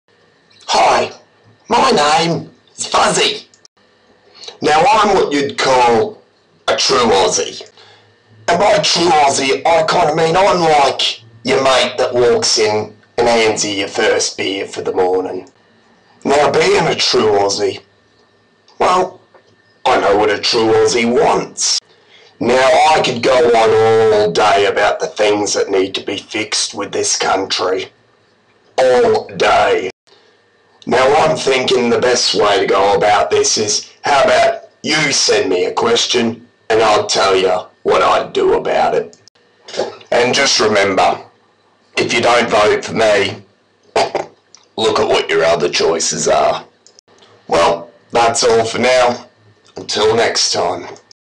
Vote 1 for FUZZIE
Fuzzie is going for Prime minister of Australia. He is a true Aussie and needs your support!